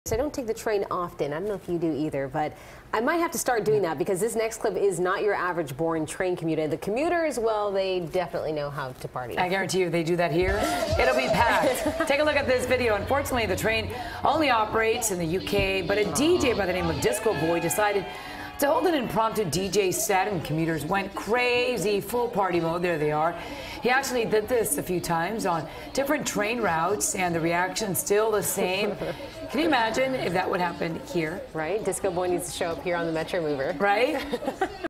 Trending: DJ Gets Commuter Train Passengers Dancing
DJ Disco Boy got the commuters into full party mode